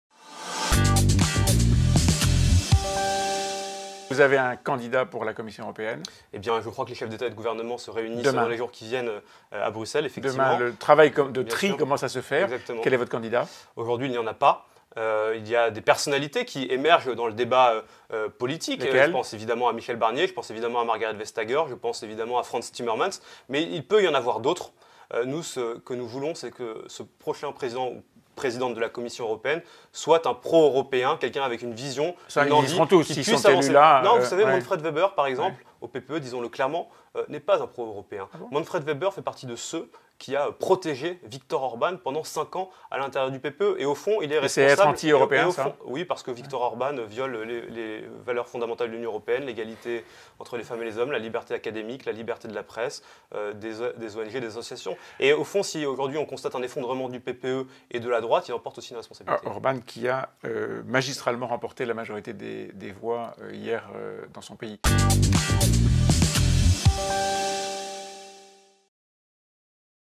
P-A. Anglade (LREM): «Orban viole toutes les valeurs fondamentales de l’Union européenne»
Quelles alliances le parti aura-t-il désormais à Bruxelles ? «Le grand enjeu de ces élections, c’était la recomposition politique en Europe (…) La co-gestion du Parlement par les sociaux-démocrates et le PPE est terminée. Désormais il y aura un nouveau groupe central, pivot, sans lequel aucune majorité ne pourra se faire (…) La délégation des députés Renaissance sera centrale, ils seront les courroies de transmission d’Emmanuel Macron en Europe qui, lui aussi, continue à être central. Avec ce groupe central au sein du Parlement, en Europe, c’est lui qui décidera !».

Et alors que Viktor Orban a remporté une large victoire, hier soir, notre invité commente: «Il viole les valeurs fondamentales de l’Union européenne ! La liberté académique, l’égalité entre les hommes et les femmes, la liberté de la presse, des ONG…. Si aujourd’hui on constate un effondrement du PPE et de la droite, il en porte aussi la responsabilité».